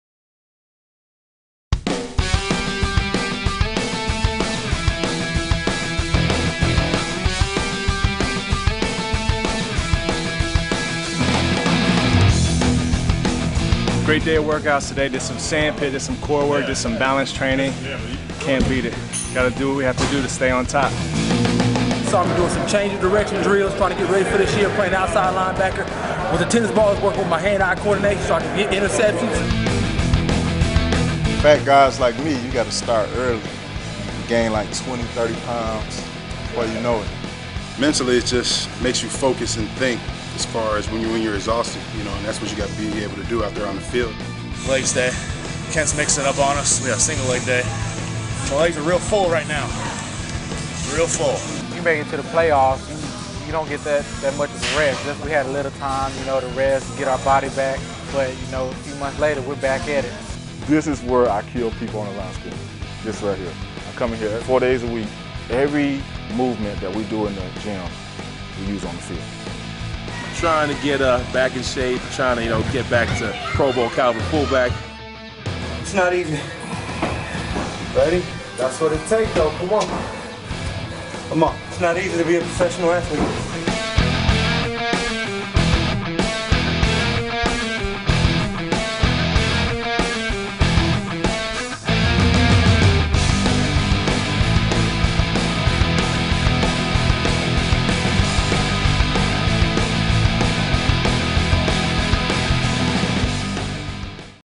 SportsBUZZ Athletes Get Ready For The Season
In this exclusive video, SportsBUZZ takes you behind the scenes as some of your favorite athletes show you what it takes to prepare for another NFL season. Get ready to sweat and for your muscles to ache.